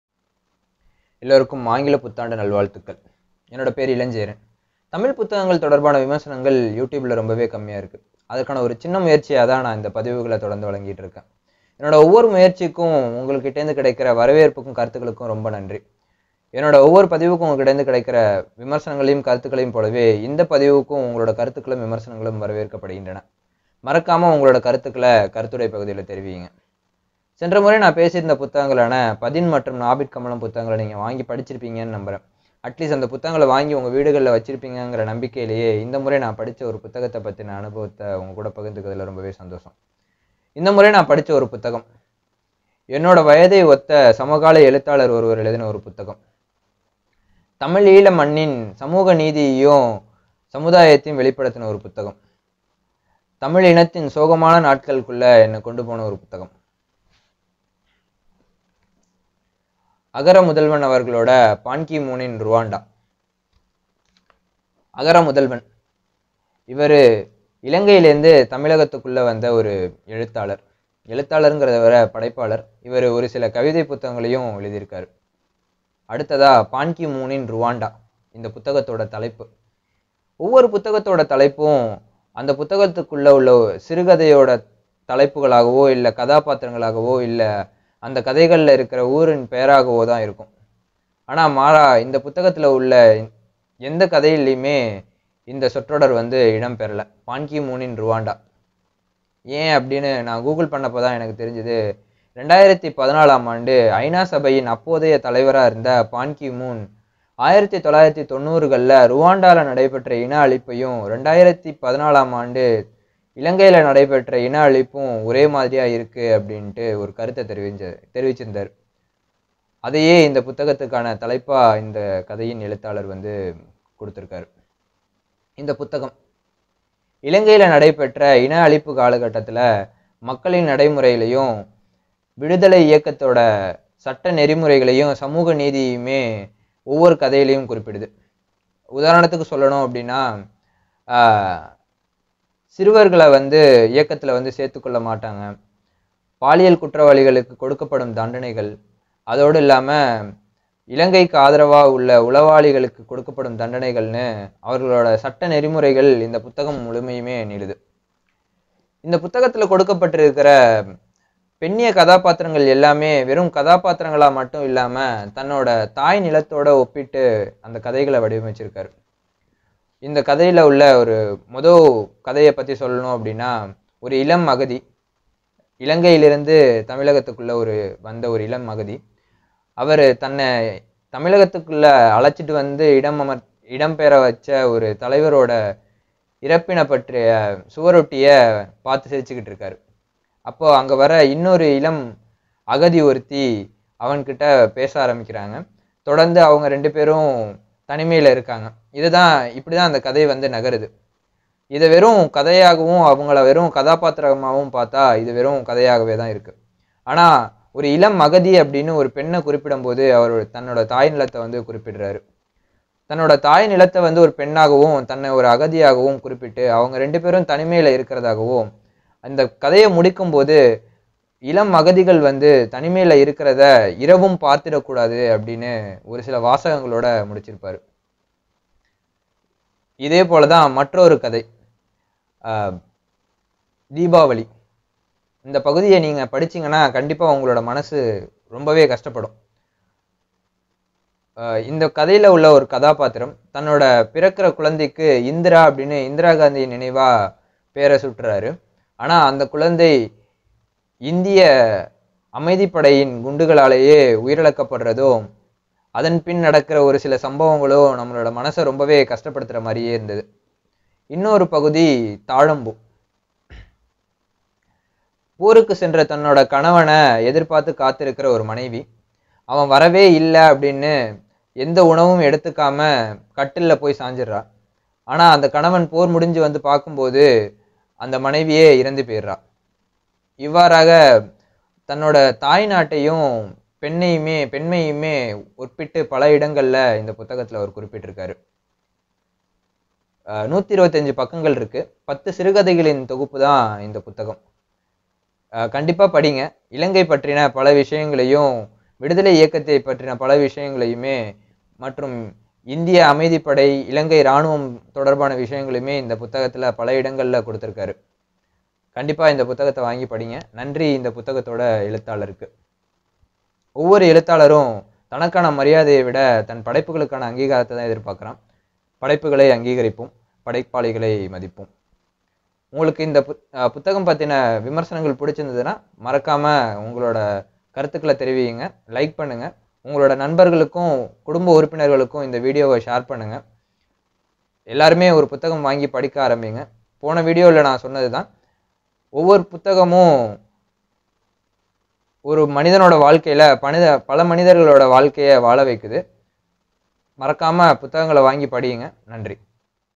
பான் கி மூனின் ரூவாண்டா | Ban Ki Moonin Rwanda | Tamil Book
In this video i have described my experience during reading this book.